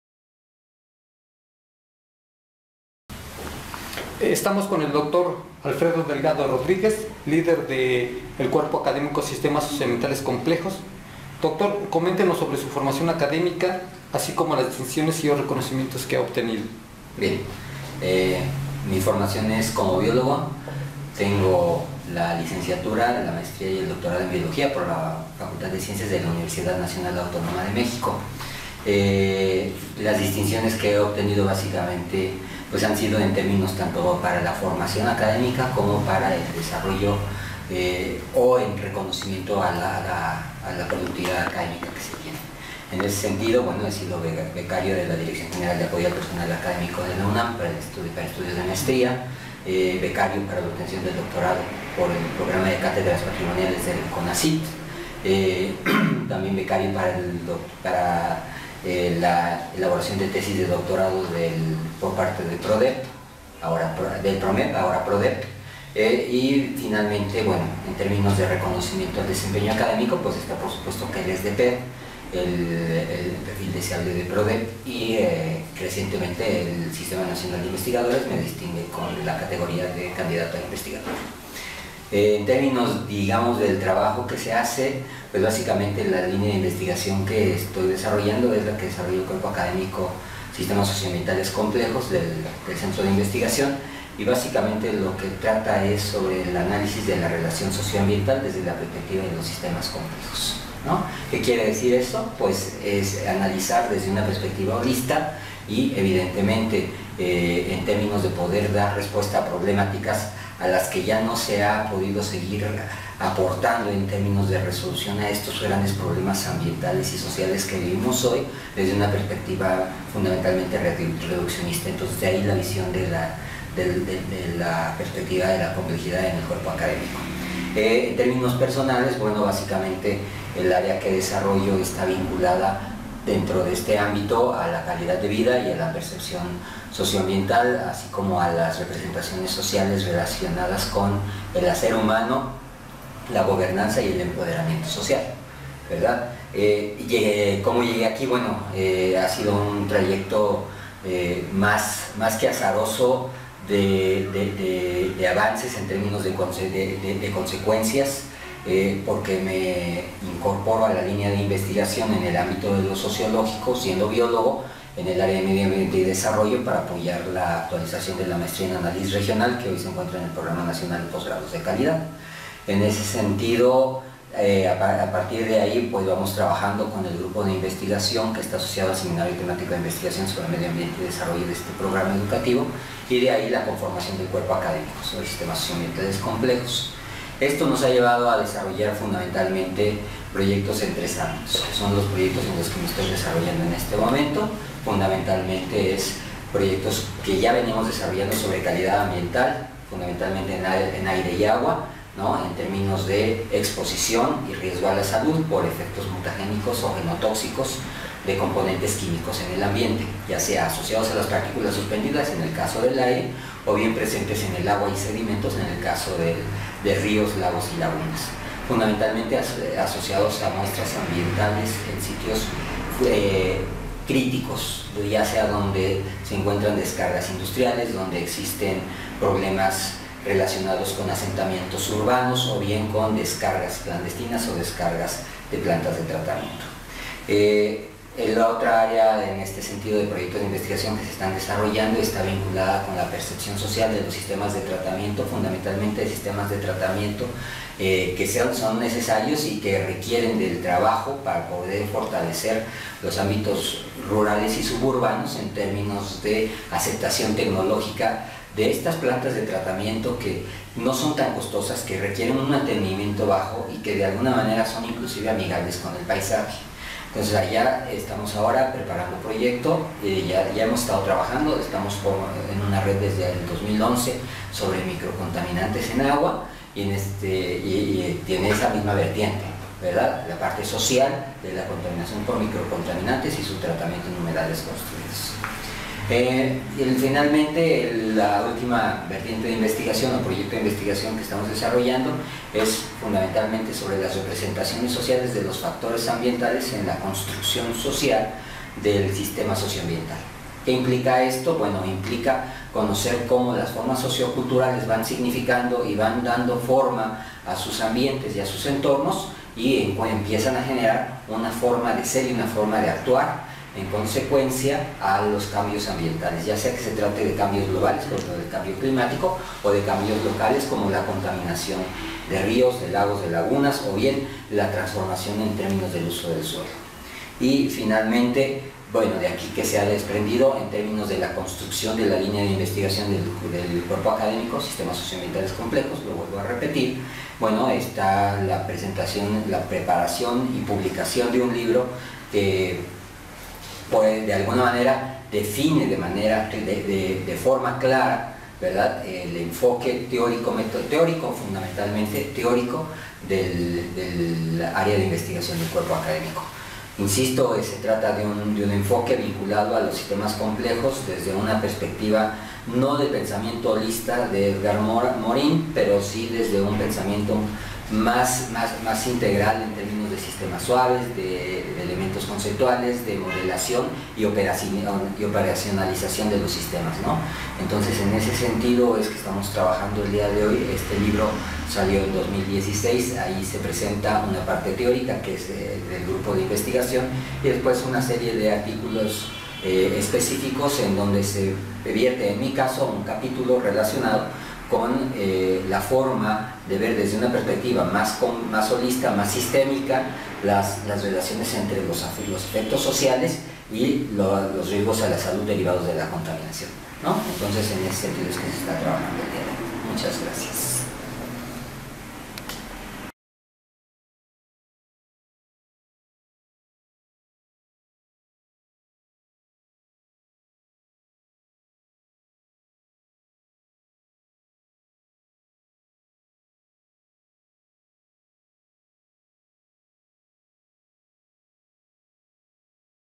Entrevista - Dr. Alfredo Delgado Rodríguez
El Dr. Alfredo Delgado Rodríguez, tiene especialidad:
Biología.
Es Doctor en Biología por la Universidad Nacional Autónoma de México (UNAM).
Maestro en Biología (Biología Celular) por la UNAM.
Licenciado en Biología por la UNAM.
Actualmente es Profesor en el Centro de Investigaciones Interdisciplinarias sobre Desarrollo Regional, de la Universidad Autónoma de Tlaxcala.
La línea de Investigación en la que se basa su trabajo es el Análisis para la Evaluación del Impacto y Riesgos Ambientales en el Desarrollo Regional
Es integrante del Núcleo Básico del Cuerpo Académico: Sistemas Socioambientales Complejos.